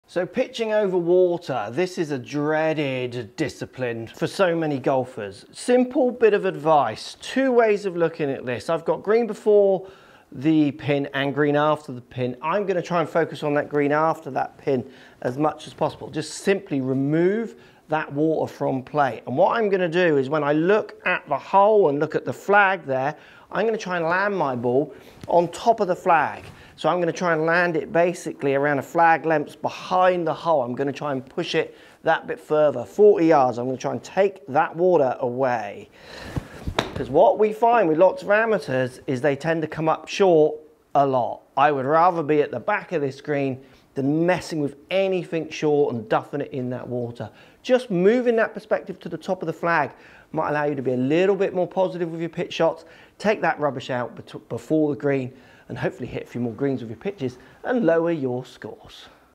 Mastering Pitching: How To Pitch Over Water | Golf Tips
Conquer your fear of pitching over water in golf by adopting a strategy that focuses on landing your ball past the pin and eliminating the water hazard from your mind. Visualise landing your ball on top of the flag to push it further and avoid falling short. By changing your perspective and aiming for the back of the green, you can enhance your pitching game and increase your chances of hitting more greens.